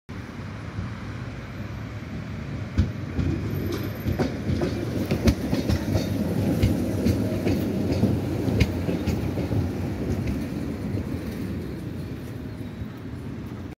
Konstal 805NaND na linii 11
Miasto: Łódź
Przystanek:Plac Niepodległości
Linia: 11
Trasa: Chojny Kurczaki - Helenówek
Data: 17.10
Numer pojadzu: 1406+1412
Informacja: Od czerwca 2022 roku wyjechali są tramwaje Konstal różnych modelów. Do tego chodzili Duewag NF6D.